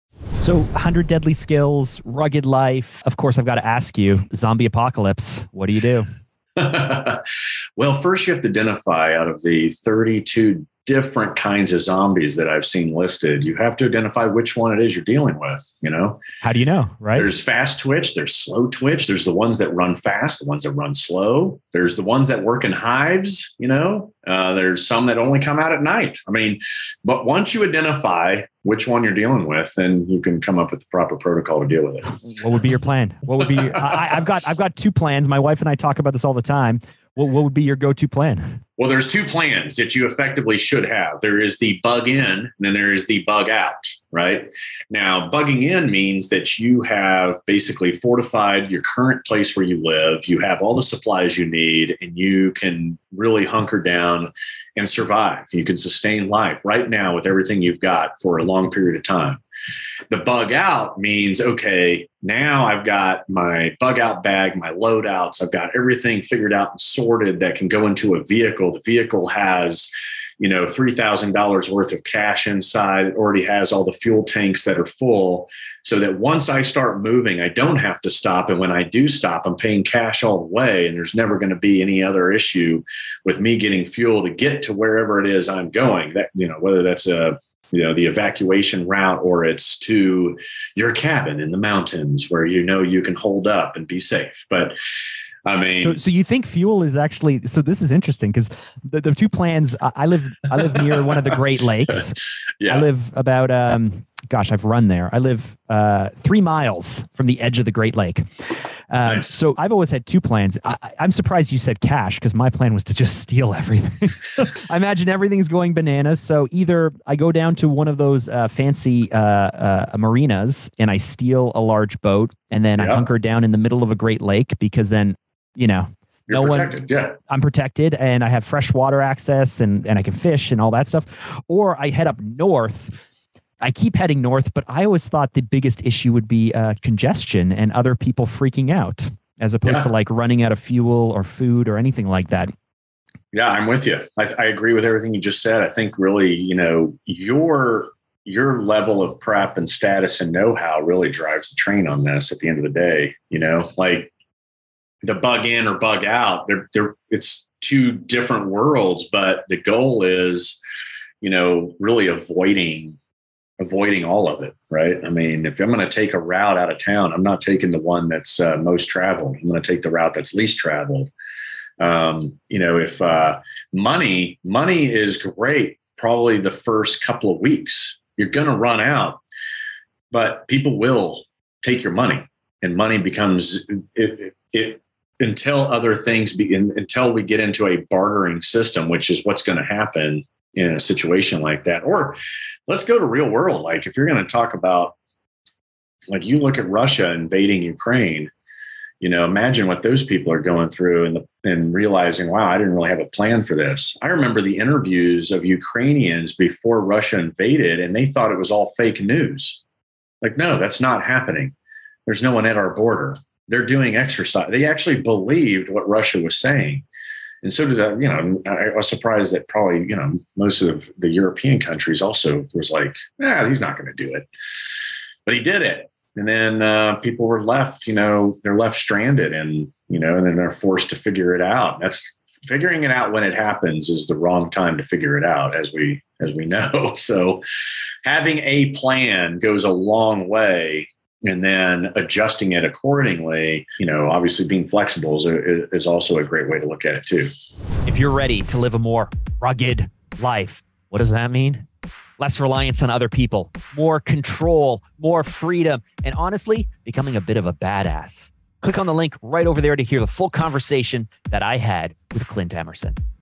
Navy Seal on How to Survive a Zombie Apocalypse
Today we talk about living a more rugged life with retired Navy SEAL, 100 Deadly Skills host, and internet survival sensation Clint Emerson.

While Clint has spent years sharing tips and tricks on how to survive some of the most life-threatening situations, covering topics like How To Escape A Kidnapping, How to Survive an Active Shooter, and How to Survive a Home Invasion, today we dig into what it takes to live a more self-reliant life.

This conversation covers why self-reliance is so important, what you can do to grow your sense of freedom, and how to survive a zombie apocalypse.